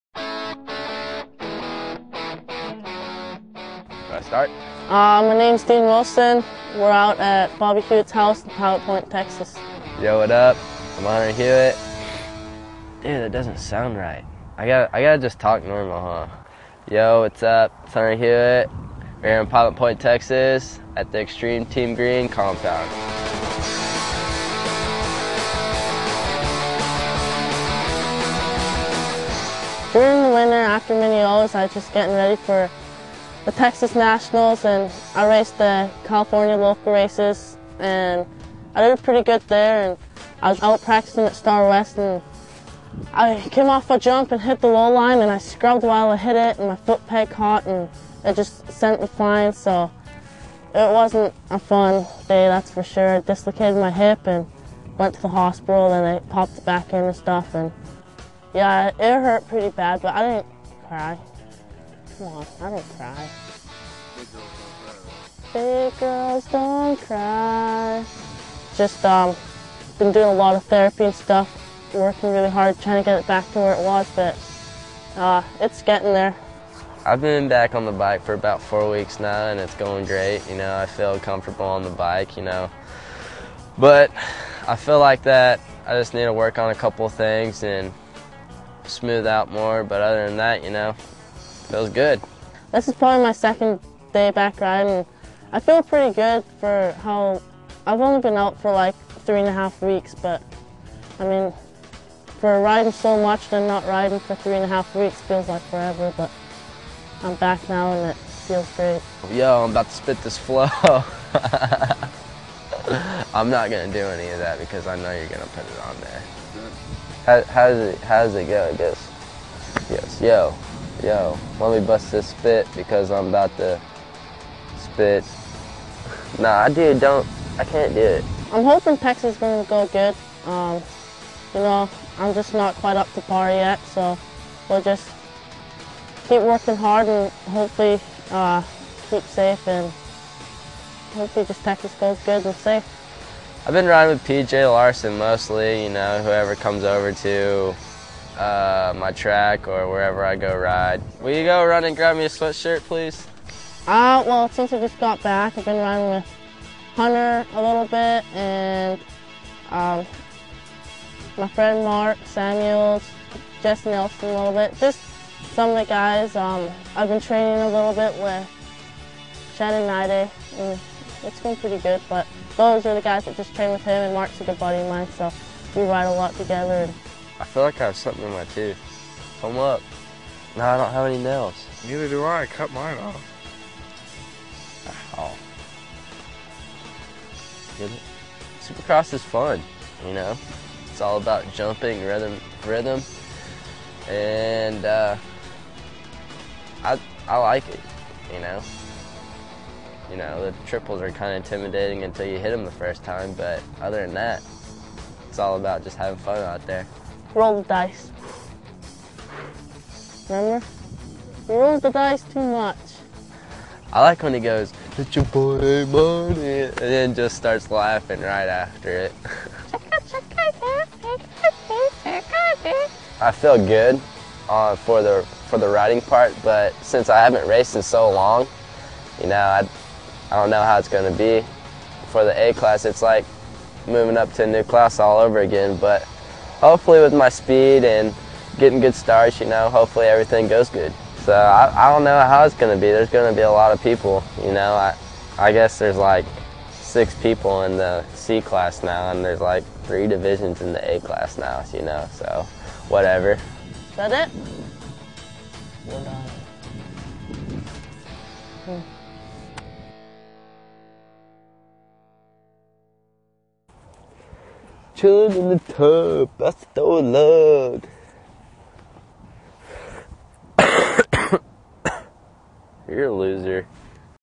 Wilson / Hewitt at XTG Compound (2008)
Highlights of Dean Wilson and Hunter Hewitt out at the XTG Compound in Pilot Point, TX before the Texas Spring Nationals. Filmed by Vurb Moto 2008